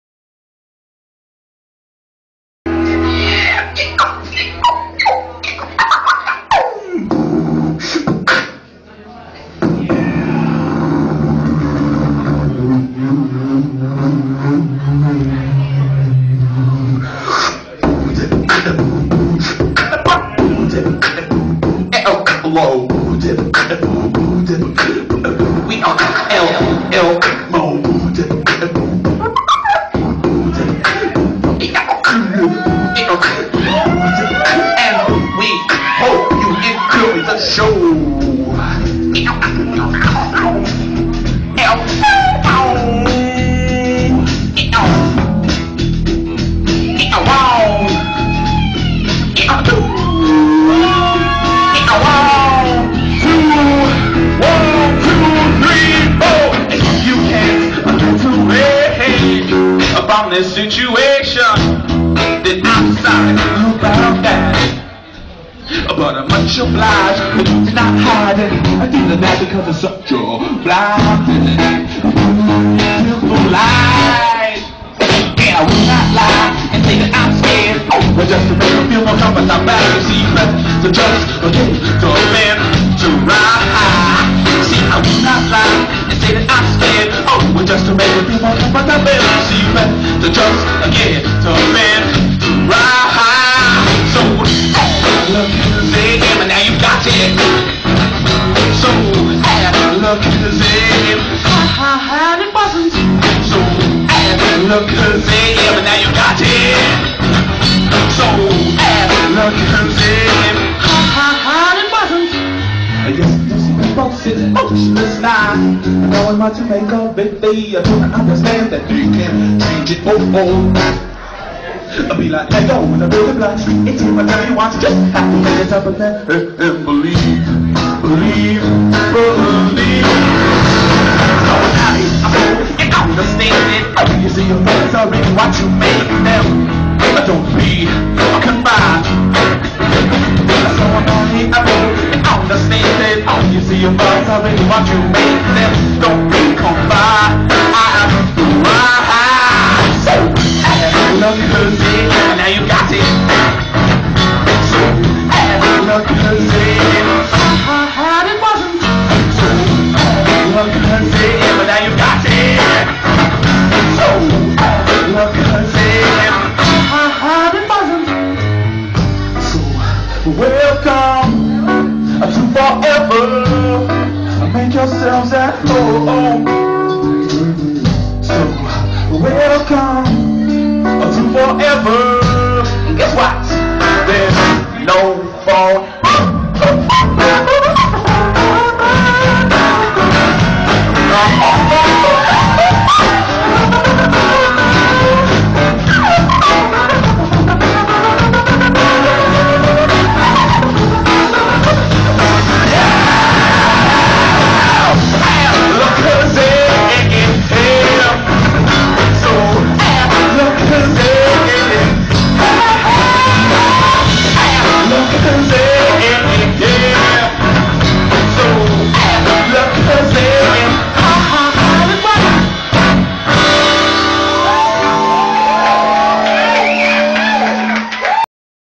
L-MO Alakazam Hull 2010.mov
Video shot by a fan in the crowd from their phone, gives you an idea as to how much fun was had on the night.

KERBLAM!